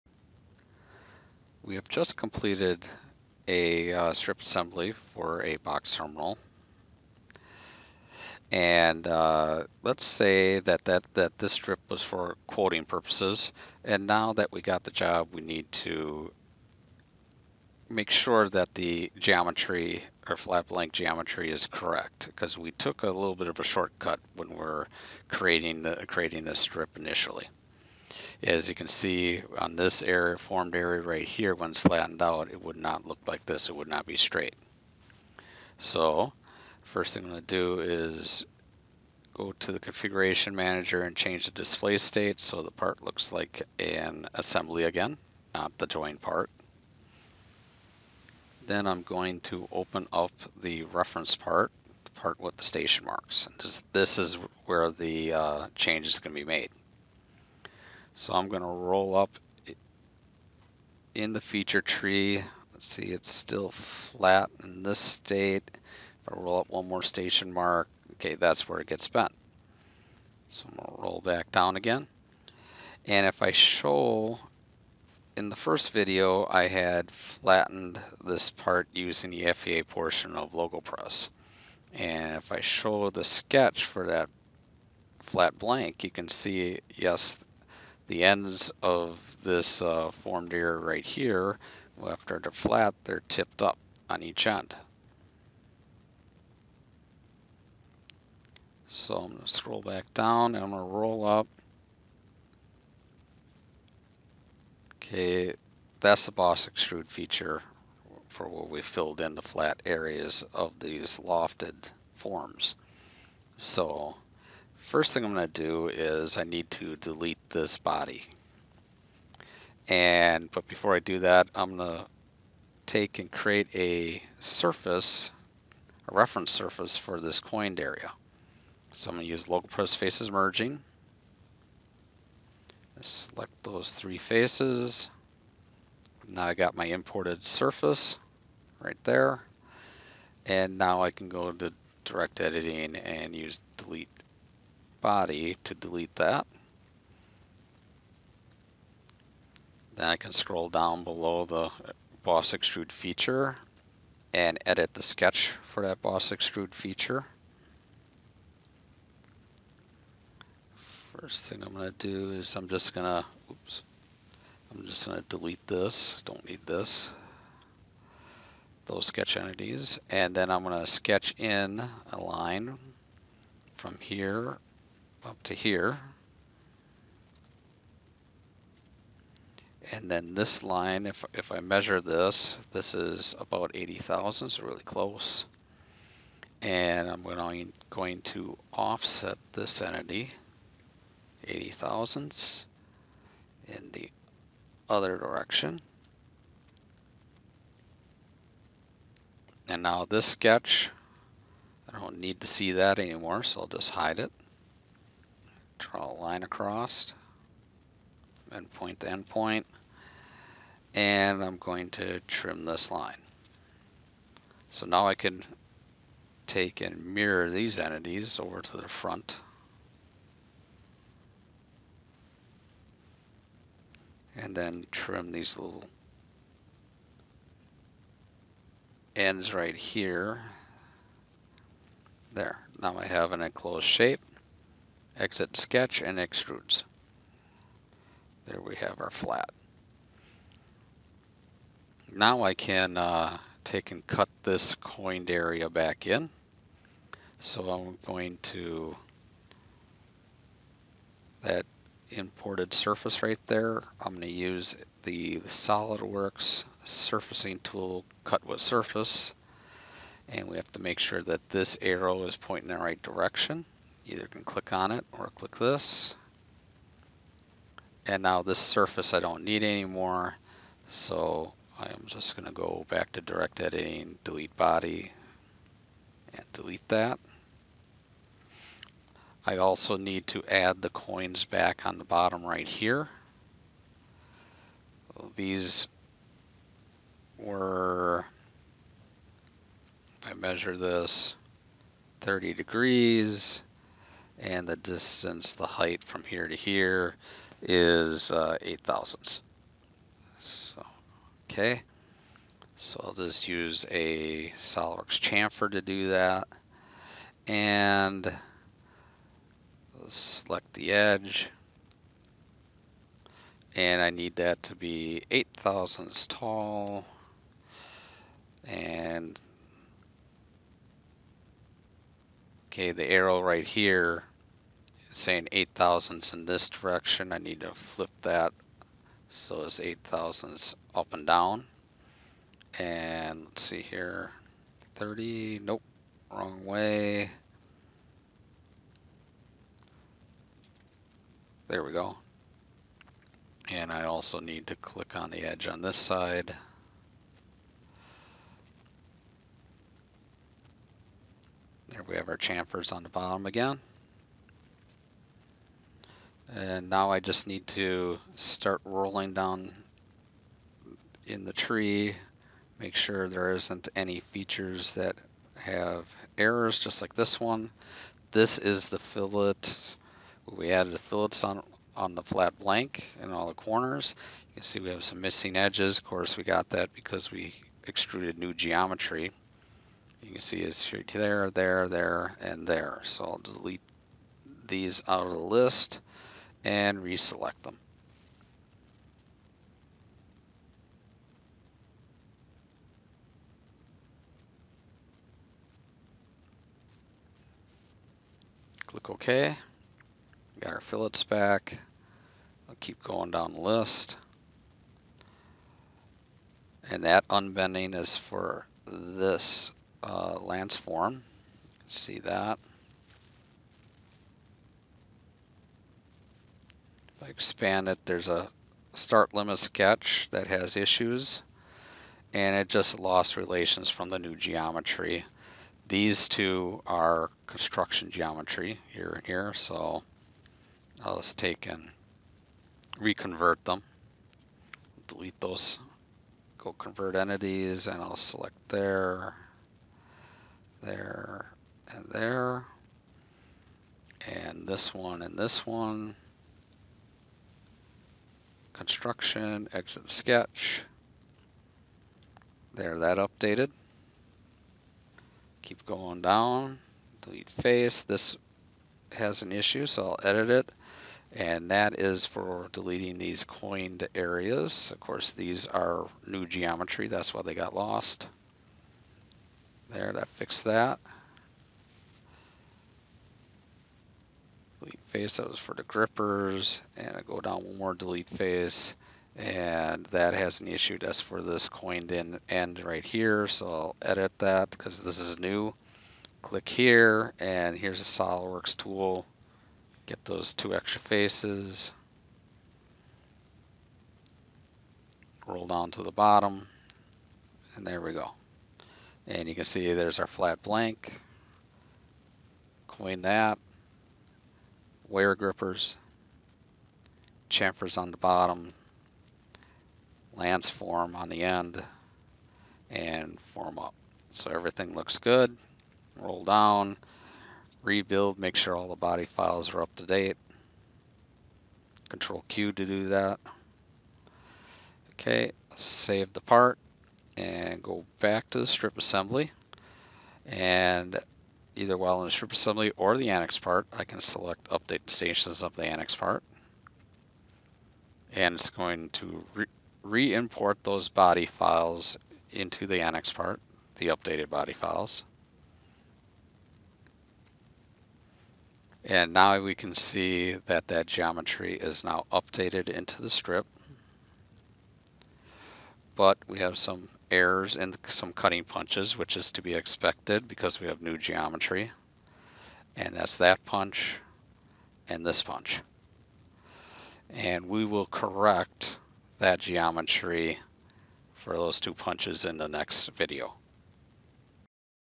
LogoPress Box Terminal Strip 2020 - 9 (First Modifications)
The main edits are done on the Reference part where we change several things in the tree so that the flat blank, particularly the crimp end, is accurately represented. After the Reference part is changed, we switch to the Annex part and it is quickly updated using the LogoPress command "Update the stations of the Annex part used in the strip". We toggle over to the strip assembly and it can be seen that this has also updated, but requires some editing or replacing of three of the punches.